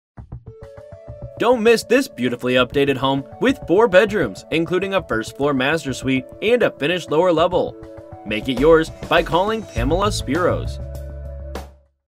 17 Rockland Road Danvers, MA | ColdwellBankerHomes.com
17 Rockland Road, Danvers, MA

Beautifully updated home with 4 bedrooms including a first-floor master suite. Finished lower level with fireplace and workshop. Large lot. Two-car garage attached with circular drive.